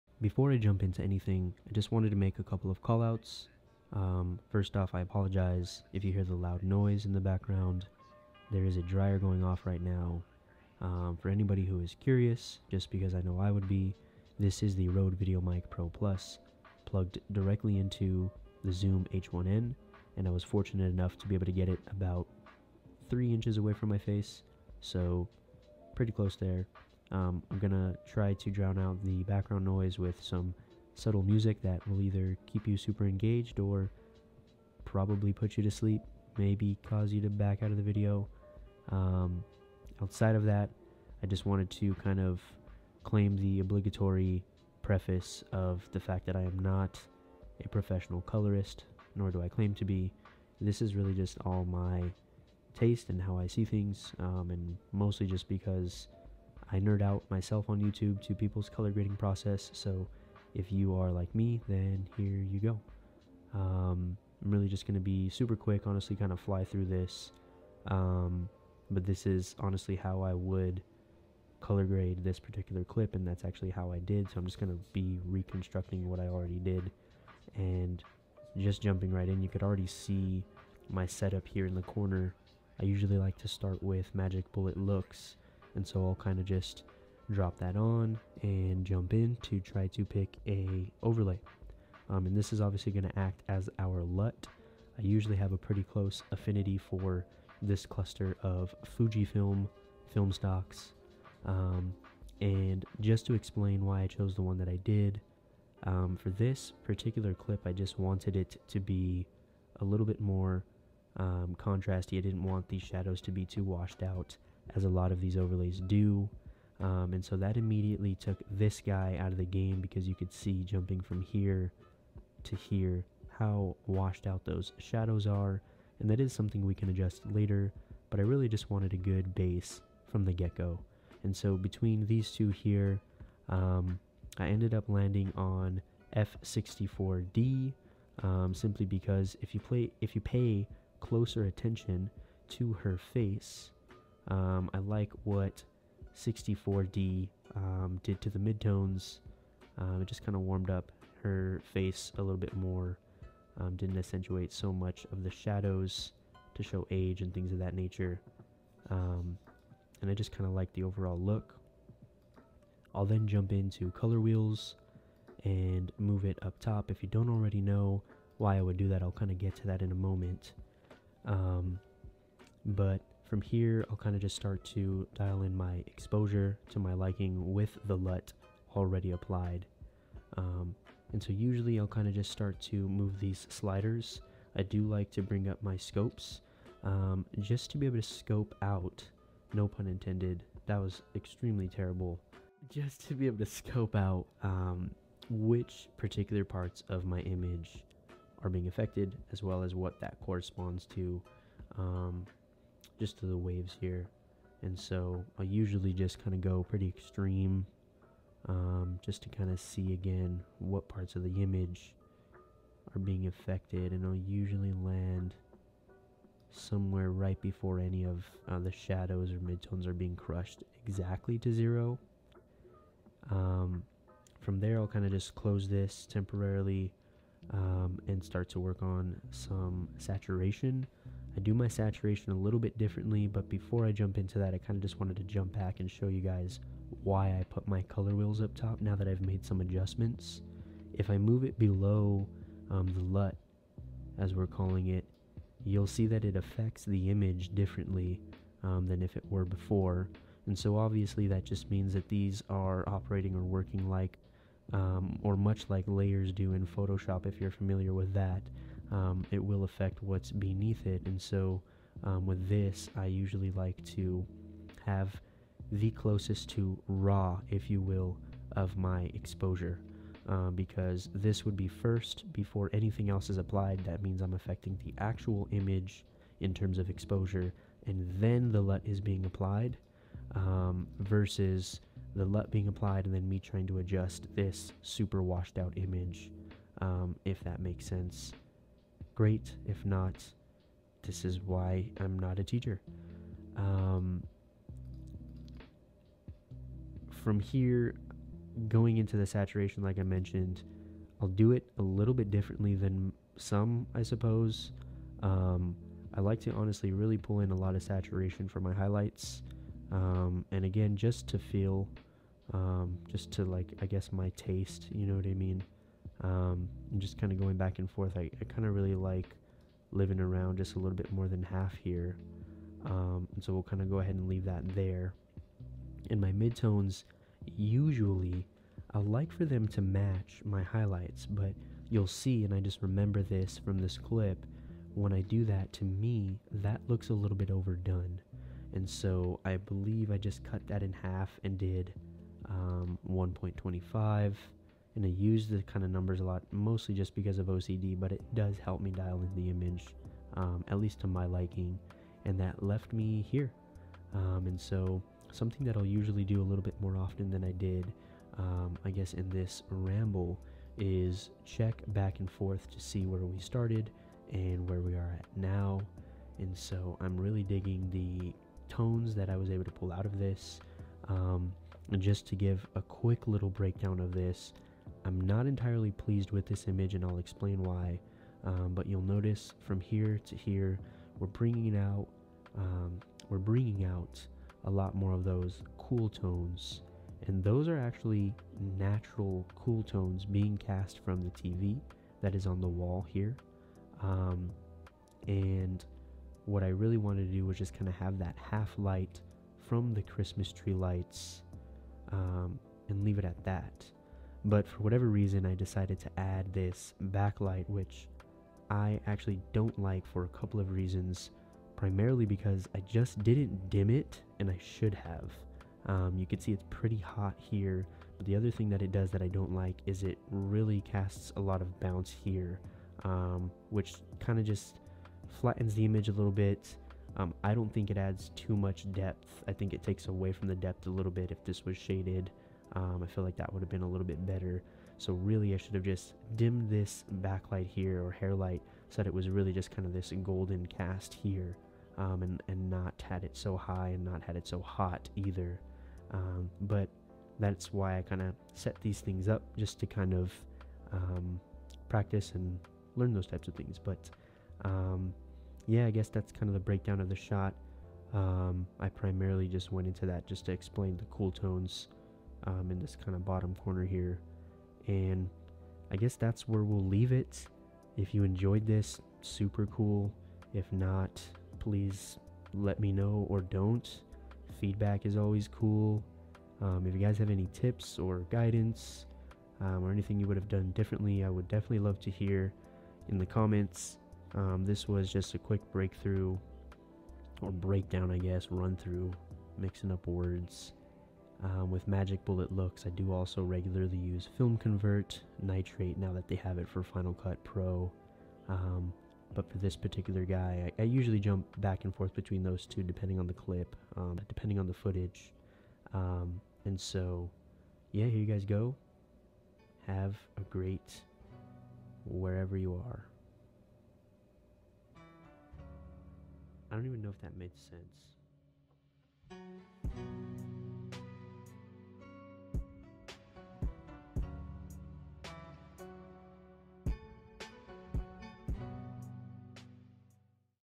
BMPCC 6K Pro Res Color Grading (FCPX)
quick run through of how i use magic bullet looks in fcpx to grade pro res footage from the bmpcc 6.